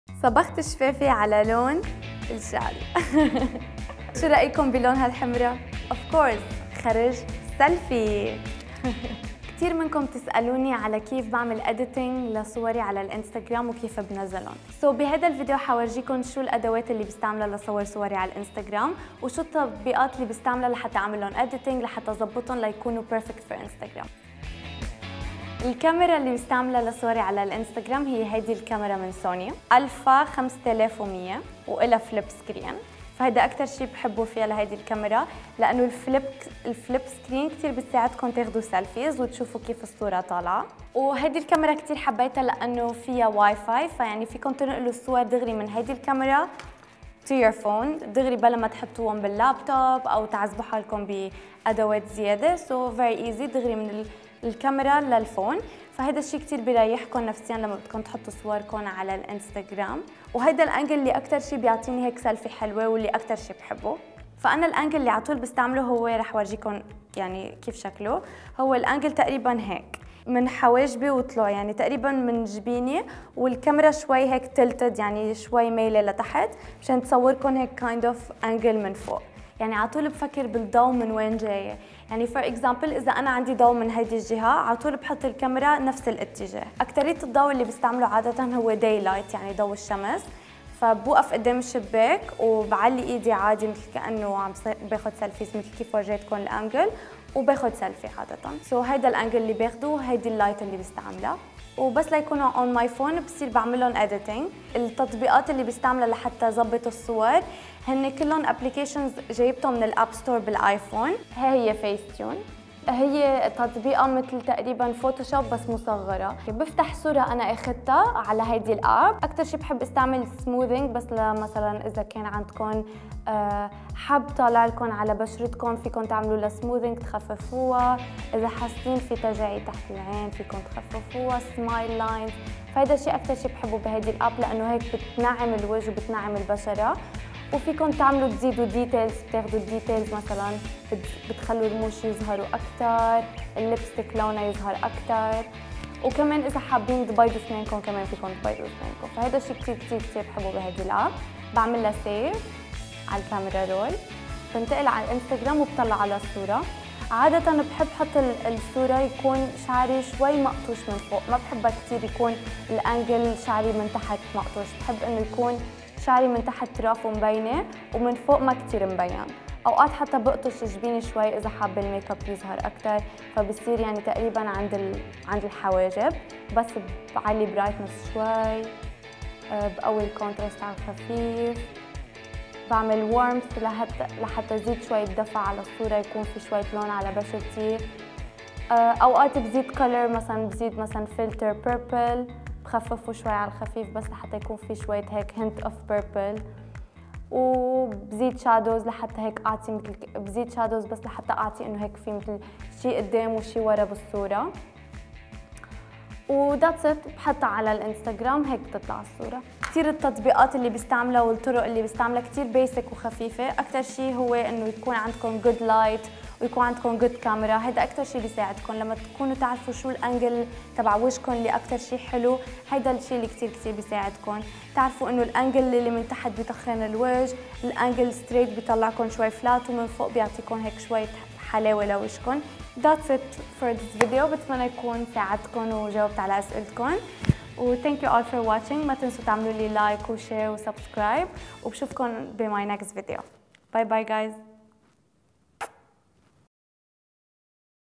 How I Take My Instagram Photos - كيفية التقاط صورة سلفي
تعلّمي كيفية التقاط صورة سلفي باحترافية من خلال هذه الحيل والنصائح.

قناة مكياج بامتياز. ساعلّمك كيفية وضع المكياج بافضل طريقة.